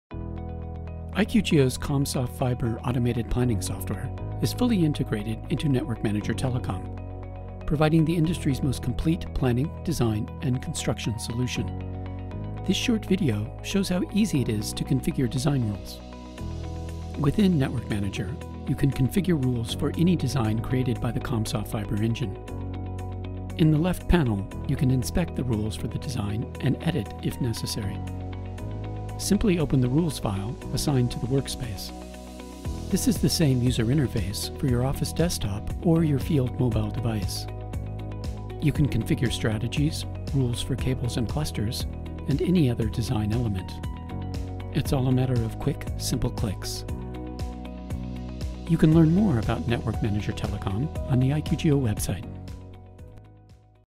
IQGeo Network Manager Telecom and Comsof Fiber integration: configuring the rules
Configuring fiber network design rules with Comsof Fiber planning integrated into Network Manager Telecom.